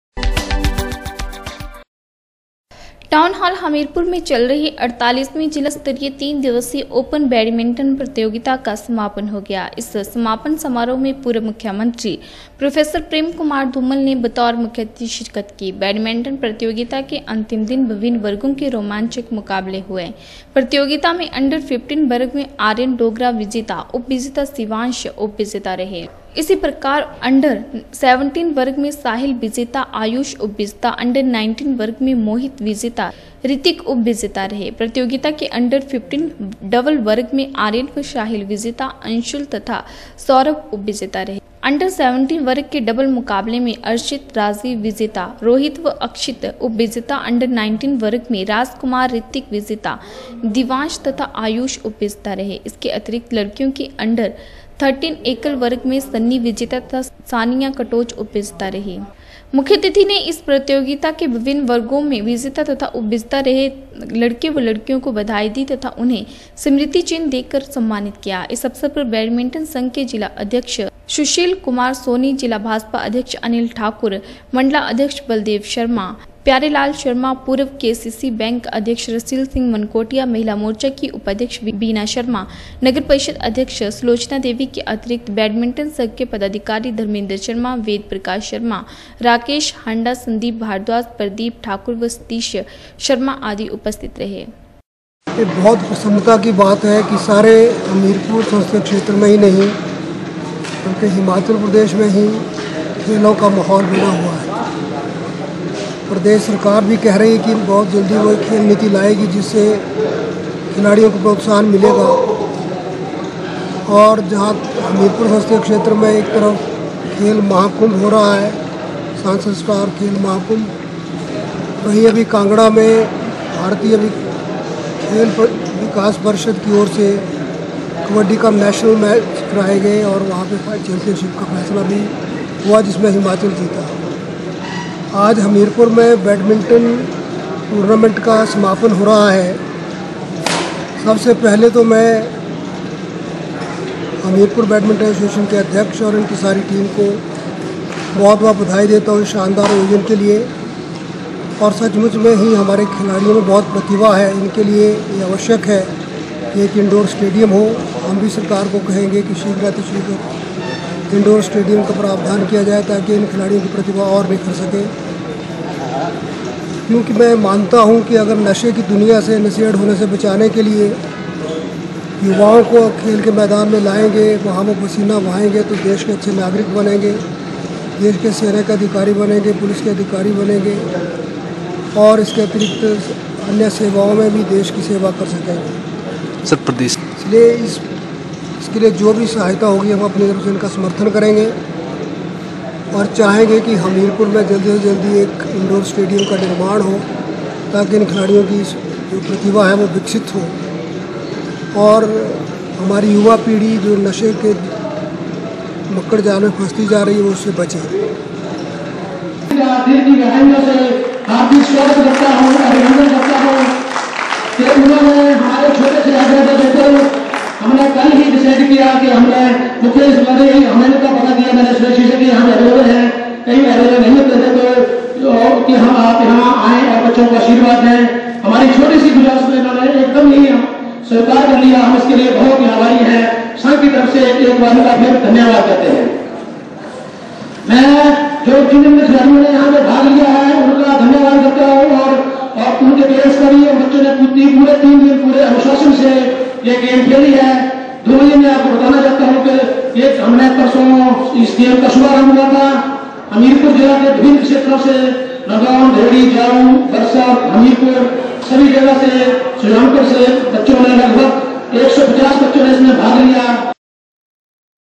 टाउन हाल हमीरपुर में चल रही 48वीं जिला स्तरीय तीन दिवसीय ओपन बैडमिंटन प्रतियोगिता का हो गया समापन ।
टाउन हाल हमीरपुर में चल रही 48वीं जिला स्तरीय तीन दिवसीय ओपन बैडमिंटन प्रतियोगिता का समापन हो गया। इस समापन समारोह में पूर्व मुख्यमंत्री प्रोण् प्रेम कुमार धूमल ने बतौर मुख्यातिथि शिरकत की। बैडमिंटन प्रतियोगिता के अंतिम दिन विभिन्न वर्गों के रोमांचक मुकाबले हुए। प्रतियोगिता में अंडर.15 वर्ग में आर्यन डोगरा विजेताए जबकि शिवांश उपविजेता रहे।